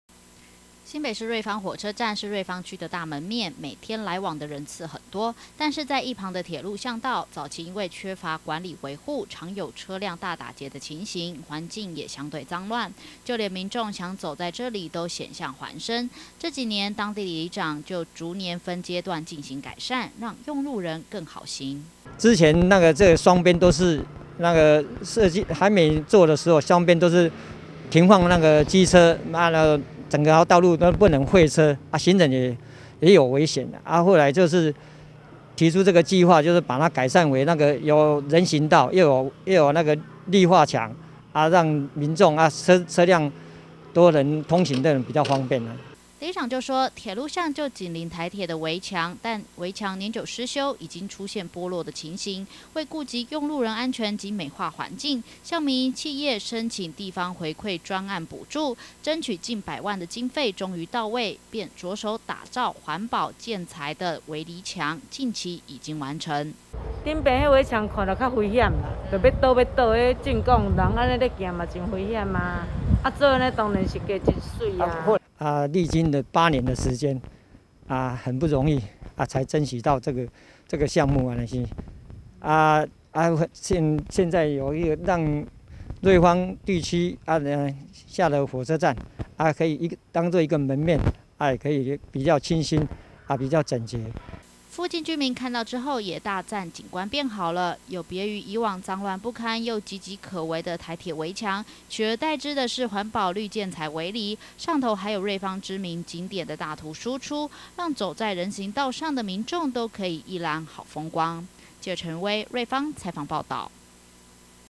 20220208觀天下新聞 瑞芳鐵路巷道圍籬美化 民眾讚景觀變好
瑞芳火車站旁的鐵路巷，早期因為巷弄狹小又停滿車輛，造成交通大打結，又因台鐵圍籬年久失修，造成環境看似髒亂，當地龍興里長黃錫基逐年改善，讓巷道變乾淨，也美化髒亂不堪的圍籬，讓民眾行經時都覺得景觀大加分。